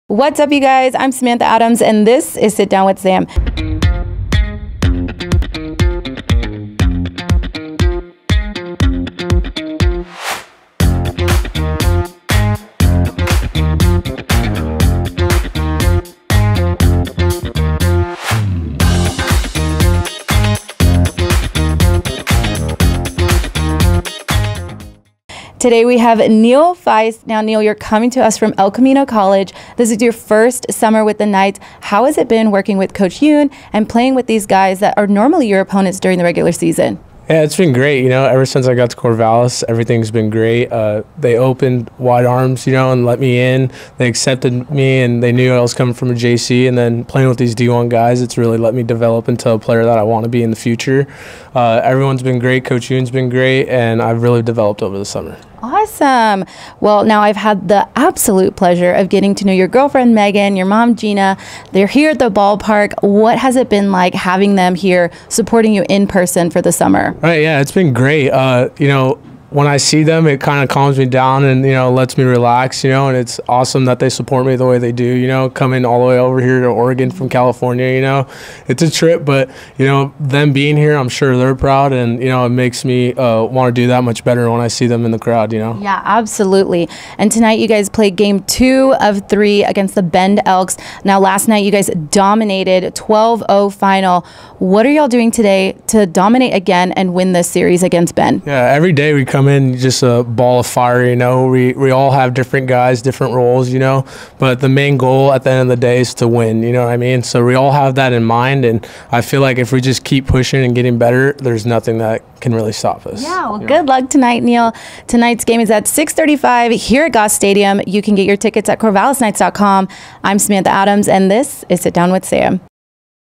Sit Down With Sam - Neil Feist
El Camino Pitcher Neil Feist sits down with Samantha Adams ahead of Game 2 of the Corvallis Knights series against the Bend Elks.